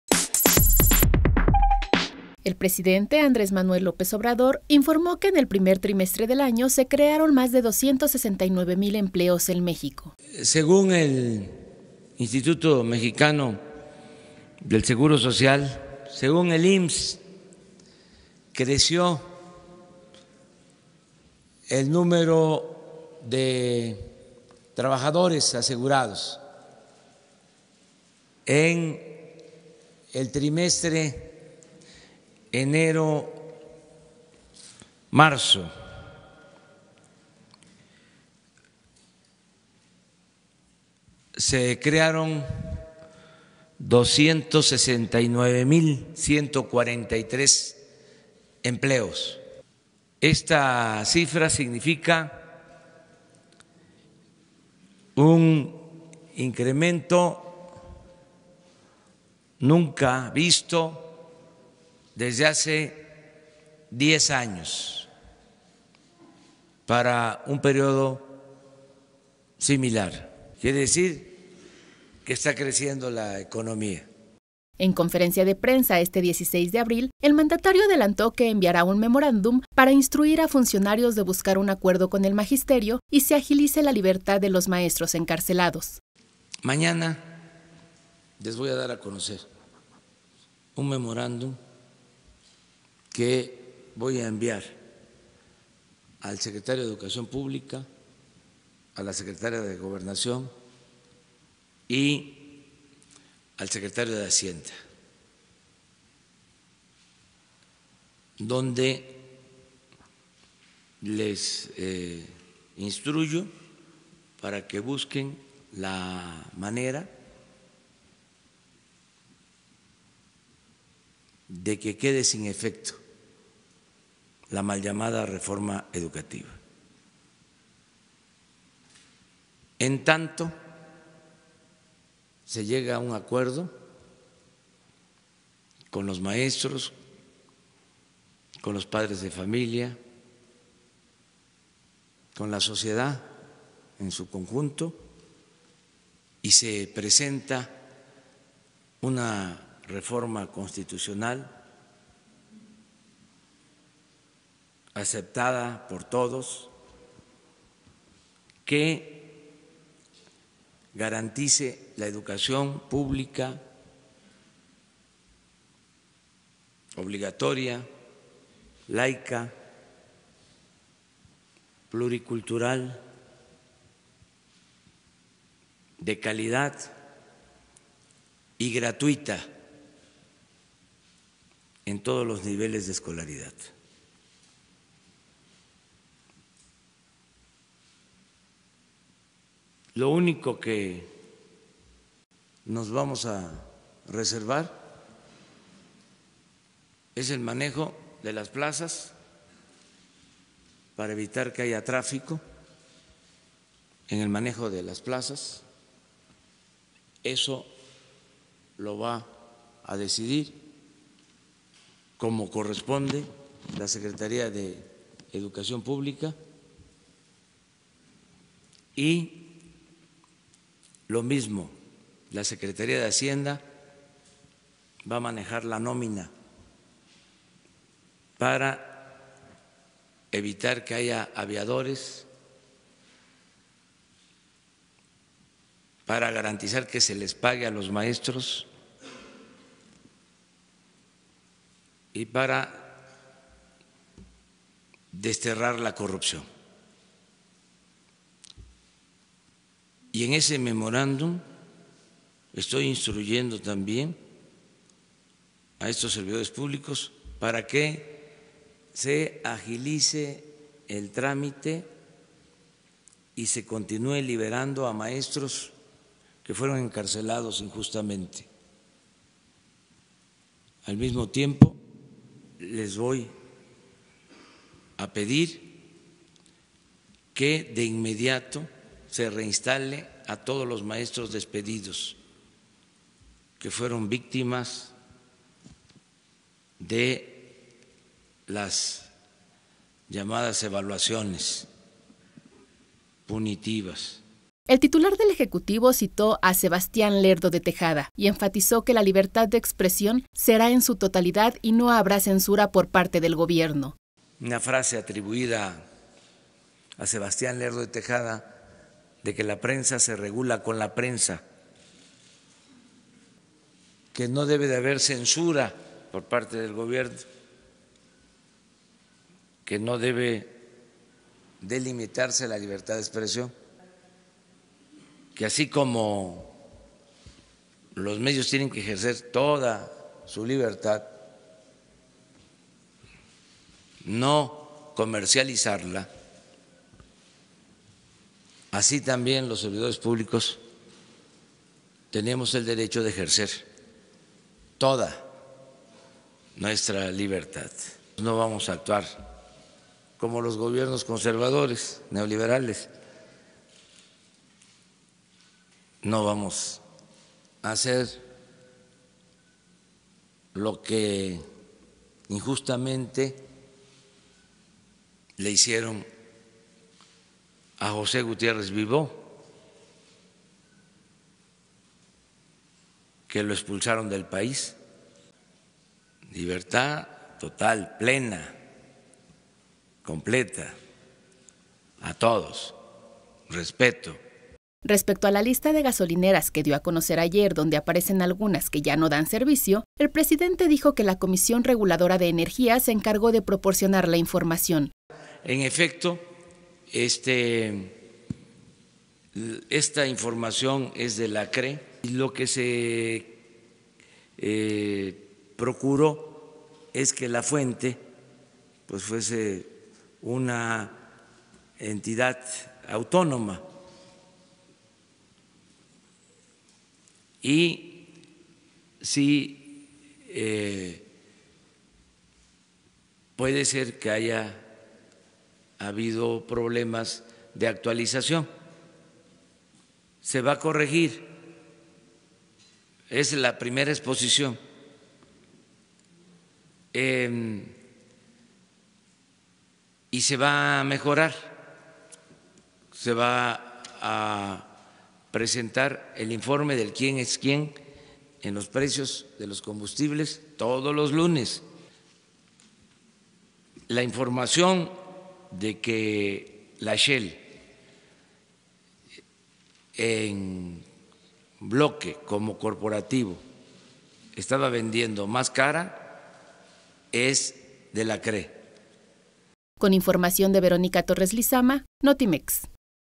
Se crean más de 200 mil empleos en primer trimestre de 2019: AMLO
El presidente enviará un memorándum para que la “mal llamada reforma educativa” se quede sin efecto y se agilice la libertad de maestros encarcelados.